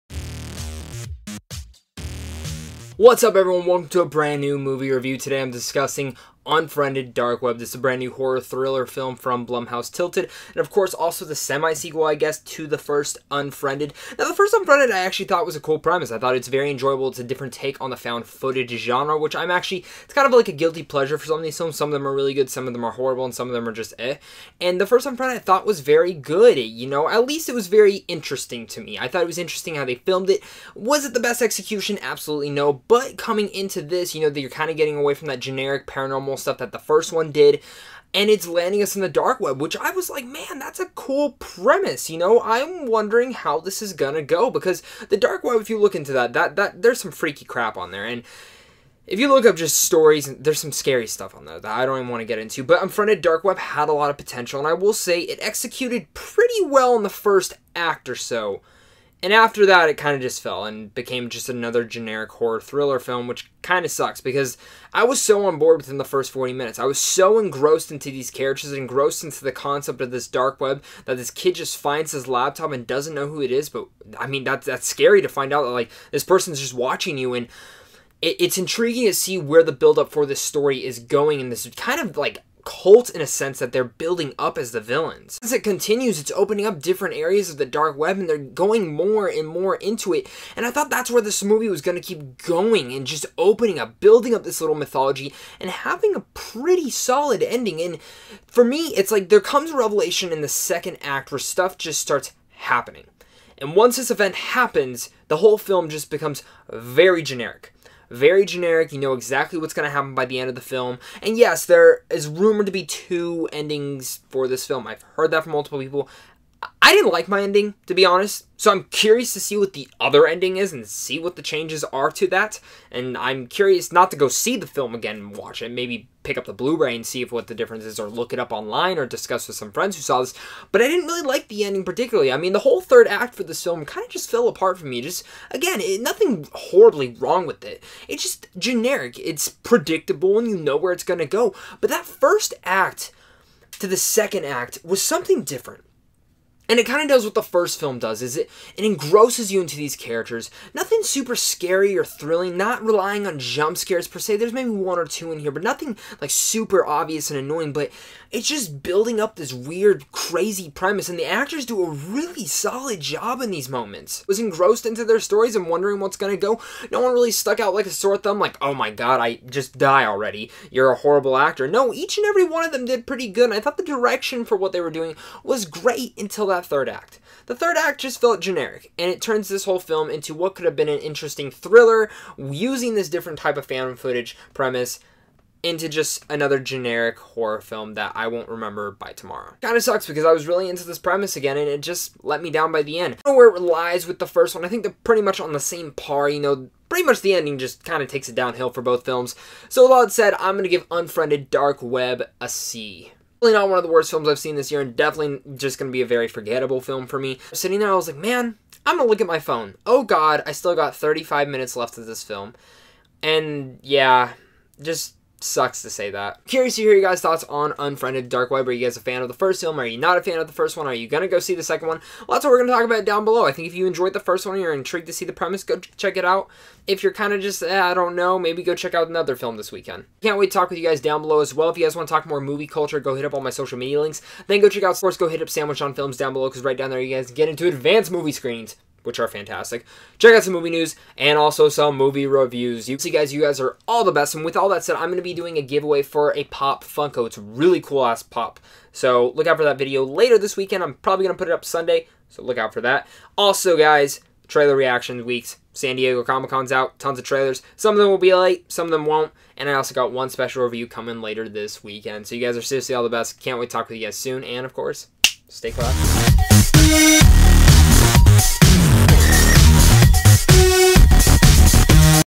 Unfriended: Dark Web - Movie Review (UNFRIENDED 2)
Synopsis: Twenty-something Matias opens up a new laptop he found in a lost-and-found bin and finds a cache of secret files on it. While on a video call with his friends, he discovers the disturbing nature of the files. They soon discover that the previous owner is aware of this and has been watching them, and they will do anything to get the laptop back.

Cast and Crew:
Directed by Stephen Susco
Produced by
Timur Bekmambetov
Jason Blum
Written by Stephen Susco
Starring
Colin Woodell
Rebecca Rittenhouse
Betty Gabriel
Andrew Lees
Connor Del Rio
Stephanie Nogueras
Savira Windyani
Cinematography Kevin Stewart
Edited by Andrew Wesman